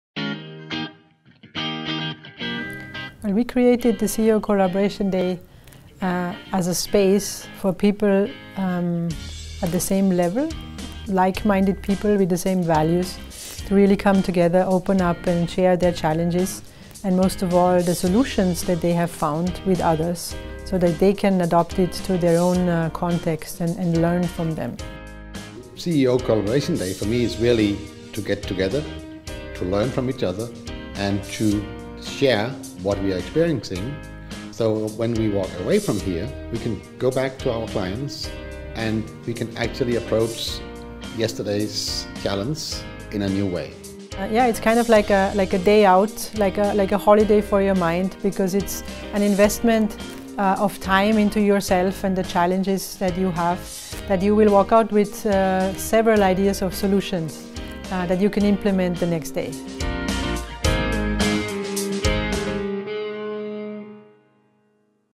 Jutta Jerlich and Jens Ulrik Hansen about the CEOCollabDay
What is the CEO Collaboration Day

With huge thanks for your true collaboration

17. April 2019